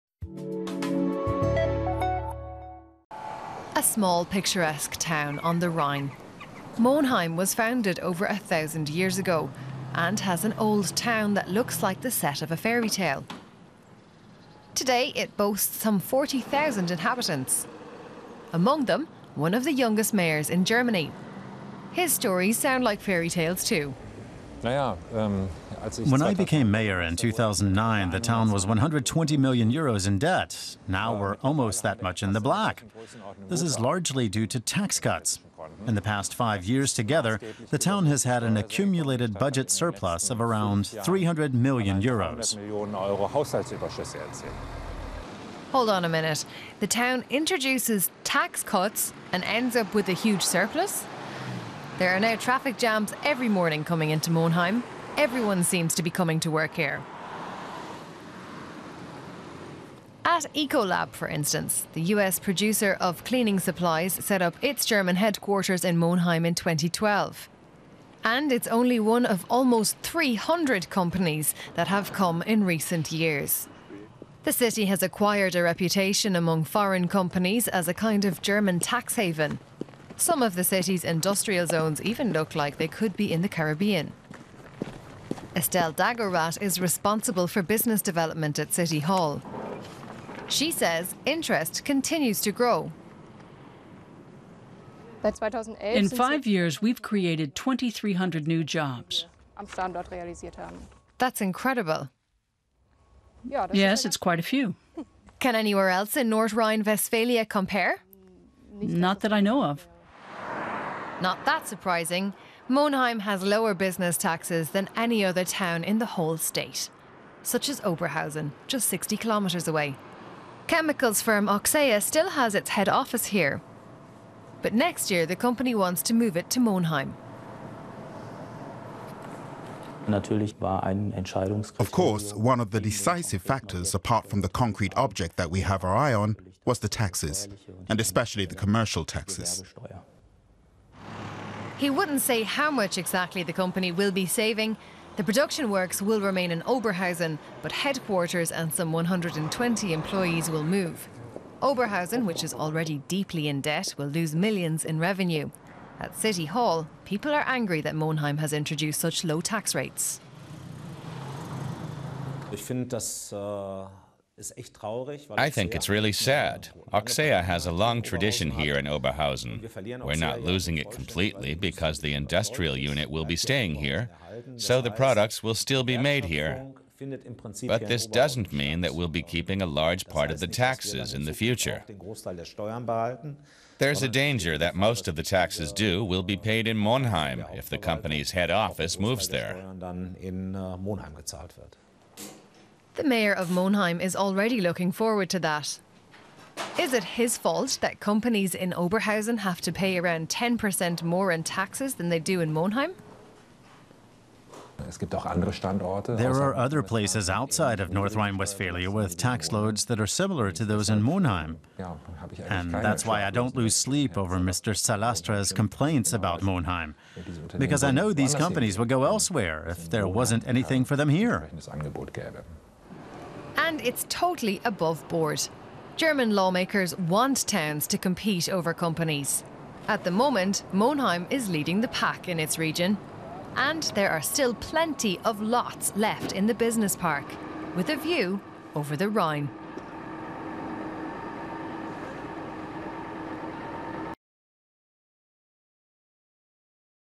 Competing taxation rates | Made in Germany
The term "tax haven” will typically have people thinking of the Caribbean, but Germany is in fact ranked 8th in the global list of shadow financing. In the fierce global competition to attract investors, the city of Monheim is among those areas in Germany eager to keep their business tax rates down.